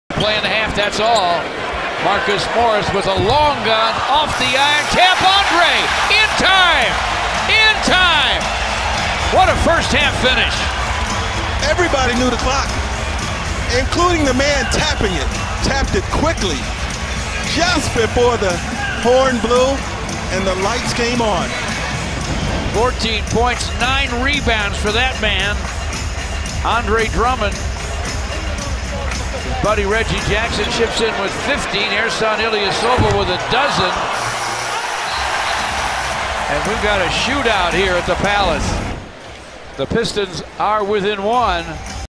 Andre Drummond Buzzer Beater Tip In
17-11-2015
Cleveland Cavaliers VS Detroit Pistons